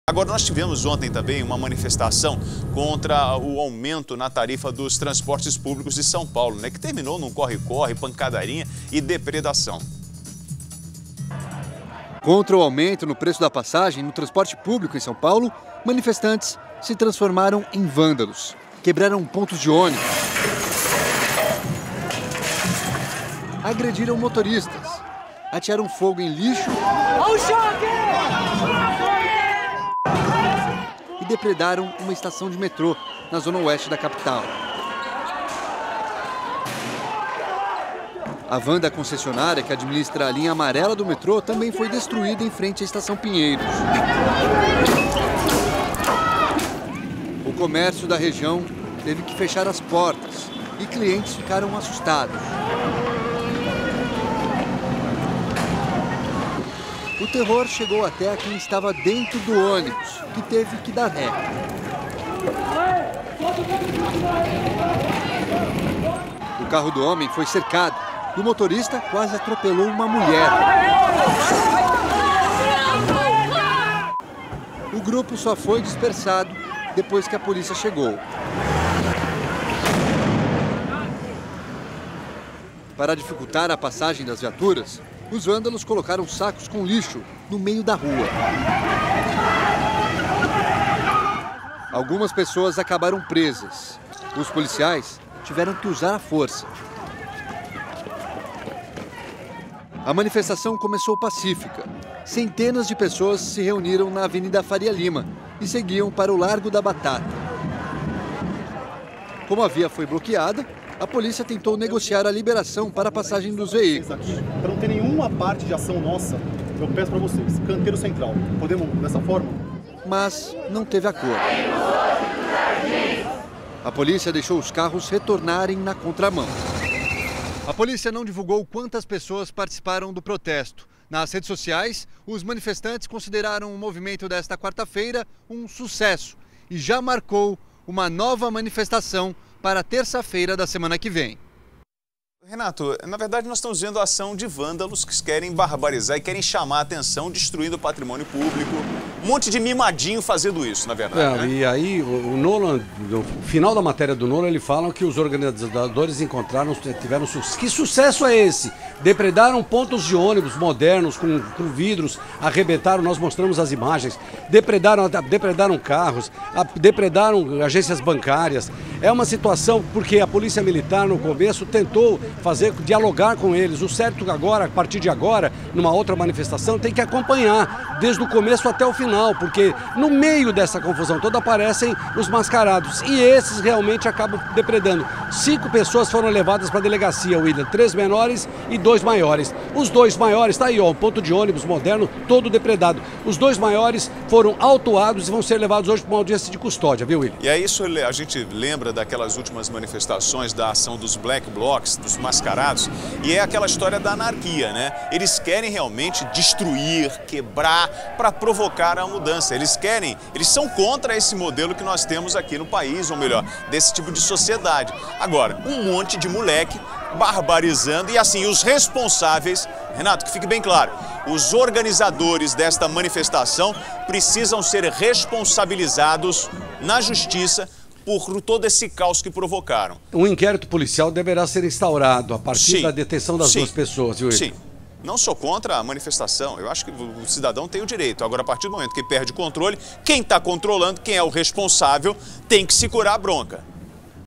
Protesto contra aumento da tarifa do transporte público acaba em pancadaria
O protesto pelo aumento da tarifa do transporte público em São Paulo (SP), acabou em pancadaria e depredação. Duas pessoas foram presas e um inquérito policial deverá ser instaurado.